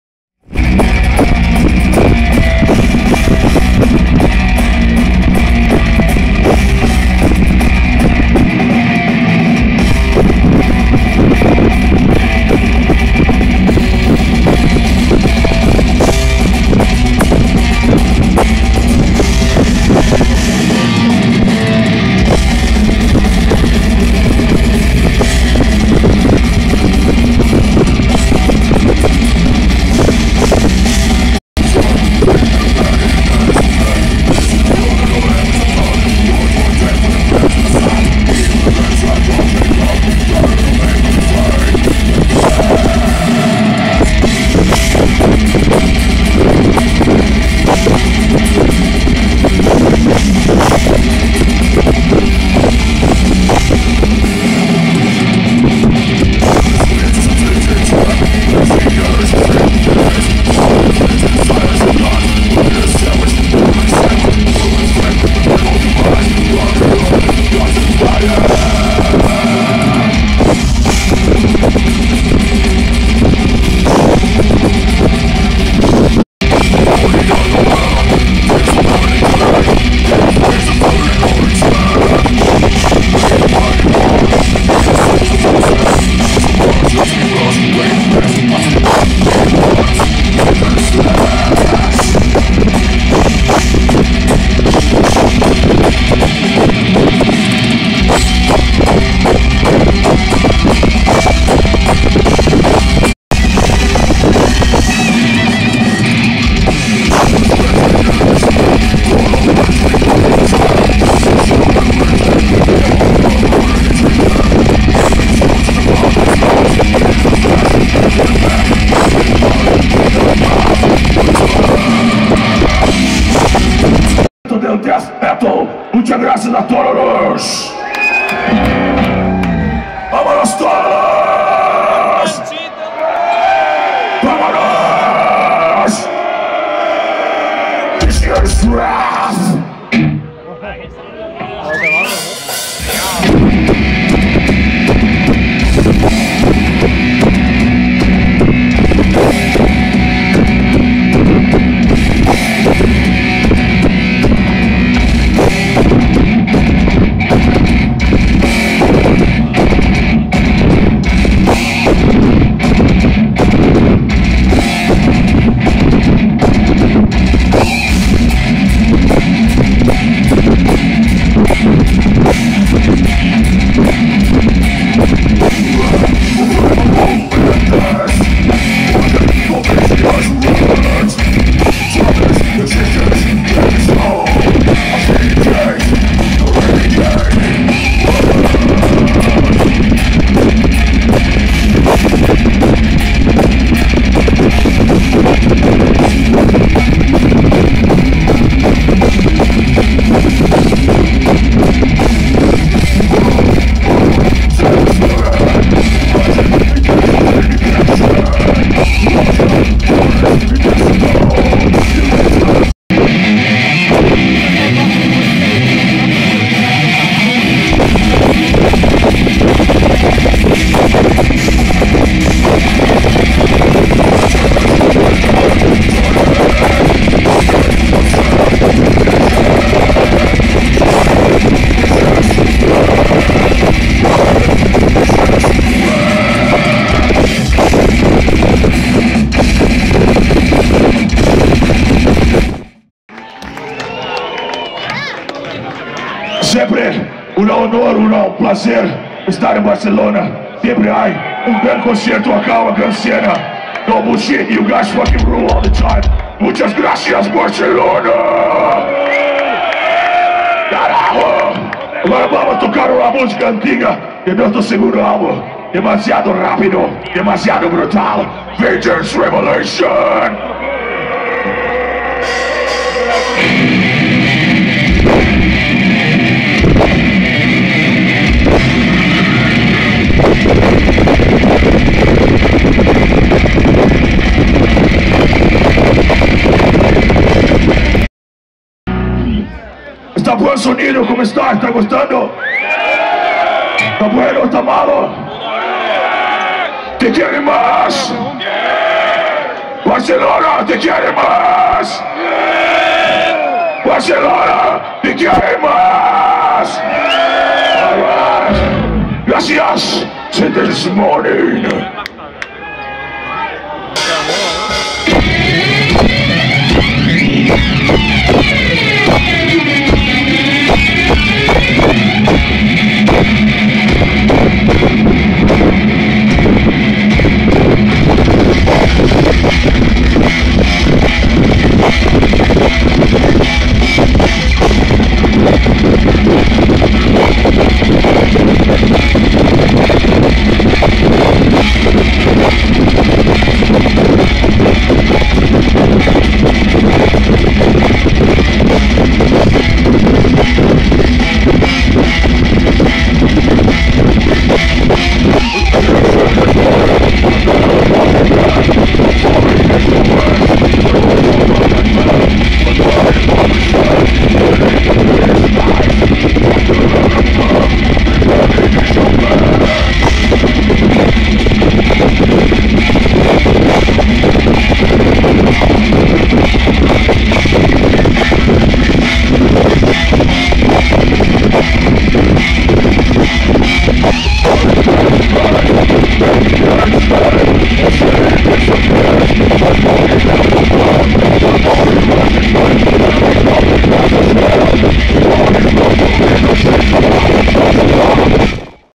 KRISIUN EN VIVO BARCELONA
*THOSE WHOM THE GODS DETEST TOUR 2009*
domingo 6 de diciembre del 2009, L'Hospitalet de LLobregat.
+NILE+
+KRISIUN+
+GRAVE+
+ULCERATE+
+CORPUS MORTALE+